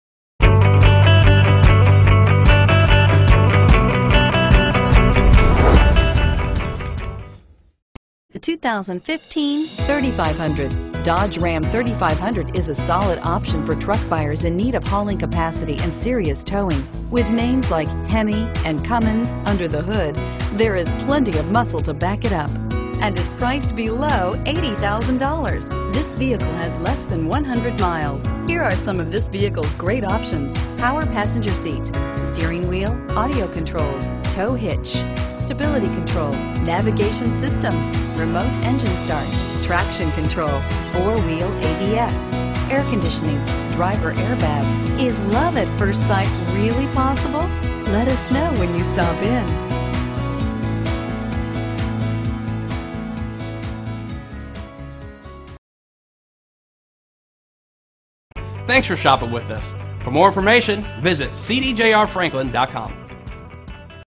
2015 Ram 3500 Franklin TN G625314
2015 Ram 3500 Longhorn 4WD 160WB

Don't miss this 2015 Ram 3500. It's equipped with Automatic transmission and features a Bright White exterior. With 4 miles, You'll want to take this car home. Make a great choice today.